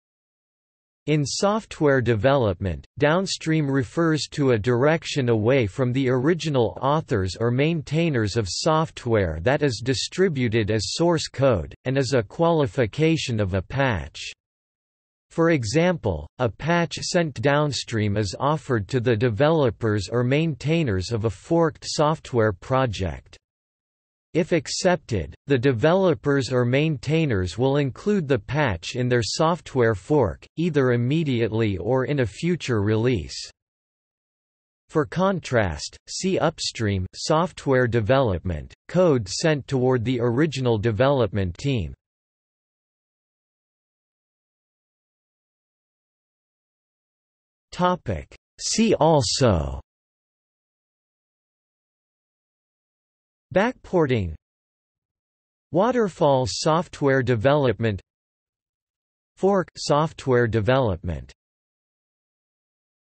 Downstream (software development) | Wikipedia audio article
00:00:44 See also

- Socrates

SUMMARY
In software development, downstream  refers to a direction away from the original authors or maintainers of software that is distributed as source code, and is a qualification of a patch. For example, a patch sent downstream is offered to the developers or maintainers of a forked software project. If accepted, the developers or maintainers will include the patch in their software fork, either immediately or in a future release.
For contrast, see Upstream (software development), code sent toward the original development team.